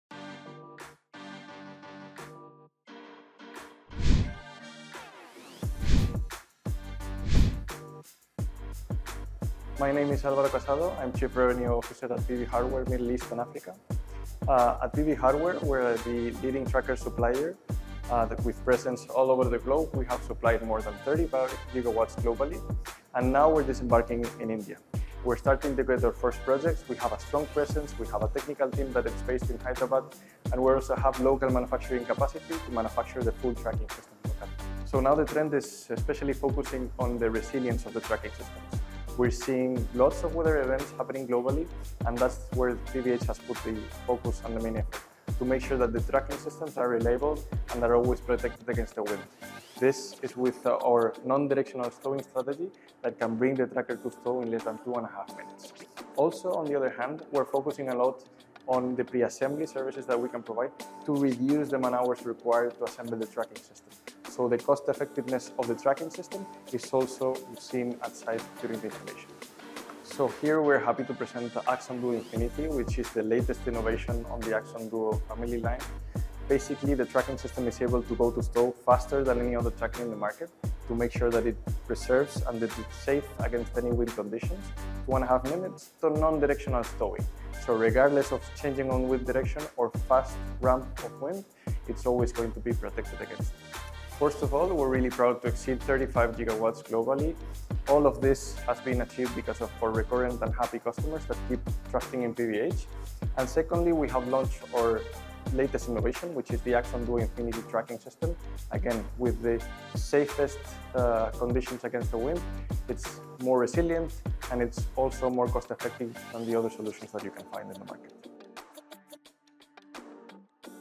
Exclusive: Álvaro Casado, CRO of PVH talks about Solar Tracking Innovations at REI Expo 2024!
In this insightful conversation, Alvaro Casado discusses the latest trends in solar tracking technology and how they are shaping the future of solar energy in India. He highlights PV Hardware’s newest product launches at the REI Expo 2024, explaining how these innovations stand apart from existing solutions in the market. Alvaro also reflects on PVH's recent achievements in the solar industry and shares how the company plans to address the unique challenges faced by the Indian solar market. Additionally, he discusses the evolving role of solar trackers in renewable energy integration and grid stability in India.